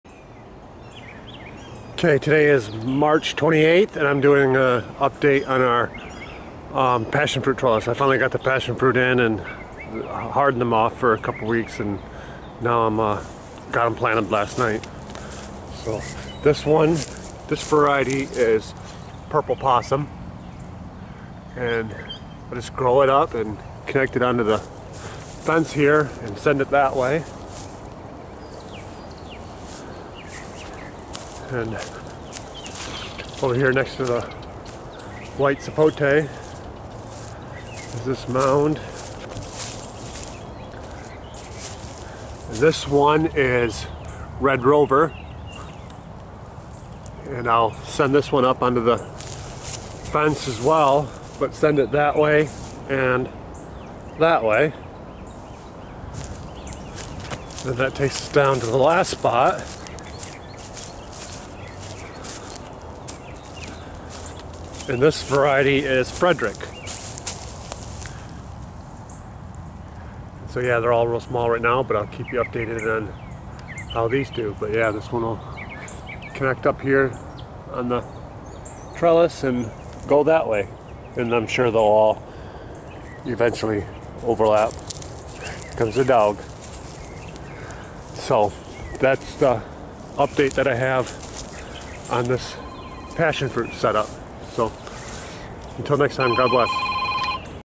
Passion fruit vines planted (Purple possum, Red Rover, and Fredrick)
This video update was done on March 28 and is showing our newly planted passion fruit varieties, purple possum, red rover, and Fredrick.